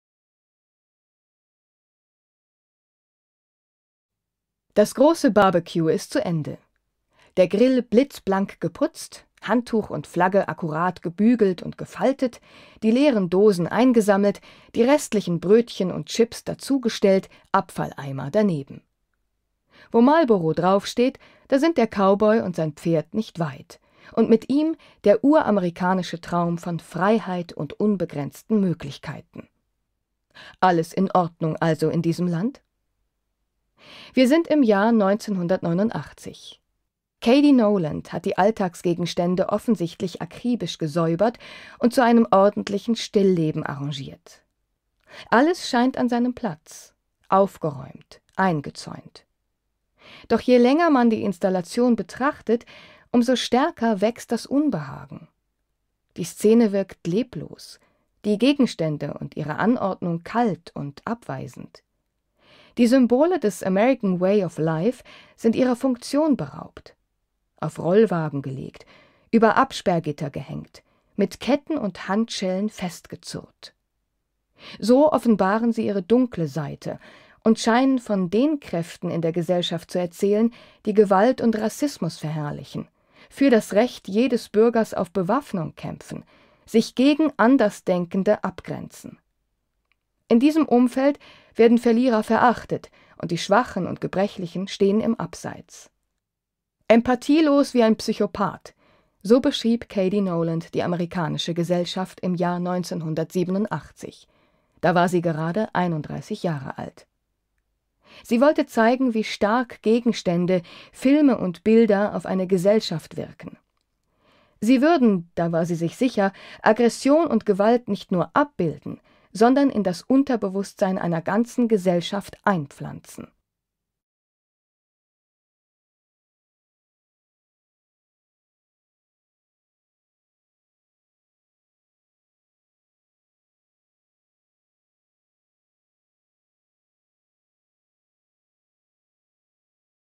MBCloseUp: Cady Noland, Deep Social Space, 1989
In der Serie MBCloseUp​​ stellen wir in jeder Folge ein Werk aus der Sammlung Brandhorst vor und geben damit einen Einblick in das weite Feld der zeitgenössischen Kunst. Heute werfen wir einen genauen Blick auf ein Kunstwerk von Cady Noland.